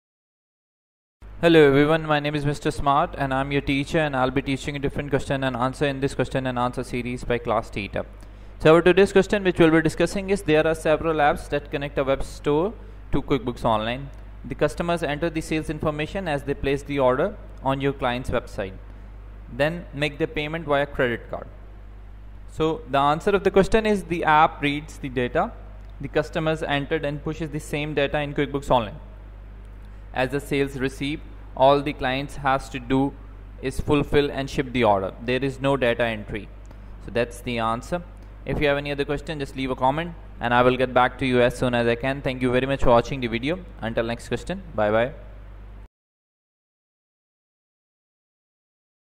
There are several apps that connect a web store to QuickBooks Online.
There are several apps that connect a web store to QuickBooks Online. The customer enters the sales information as they place the order on your client's website, then makes their payment via credit card.